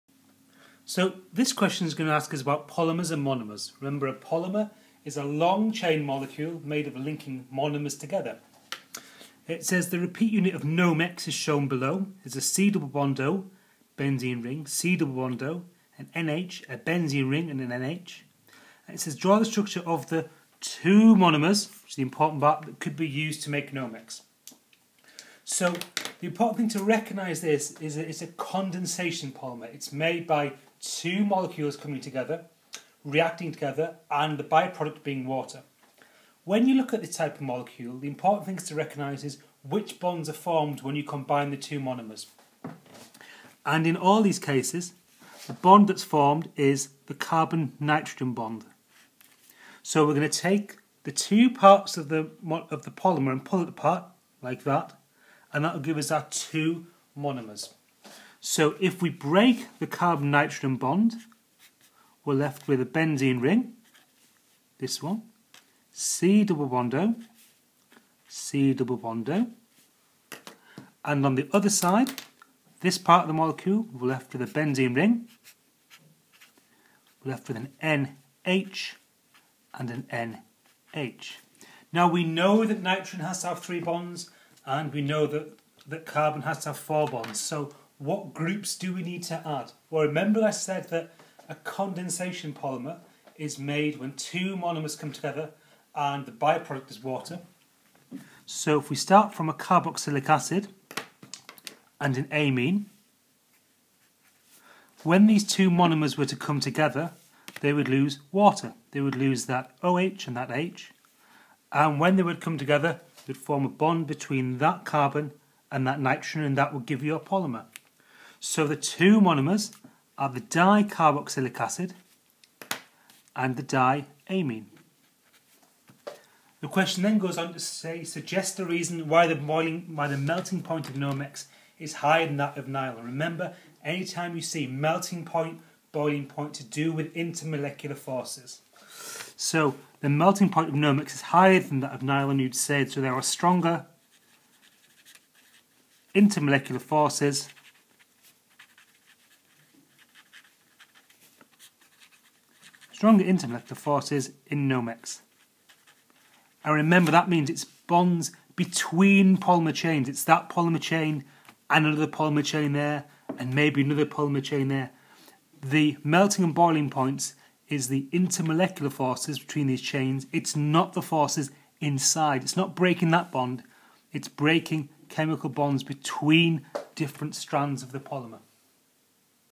How to work out the monomers that make a polymer
A worked example of how to work out which monomers make a condensation polymer, Nomex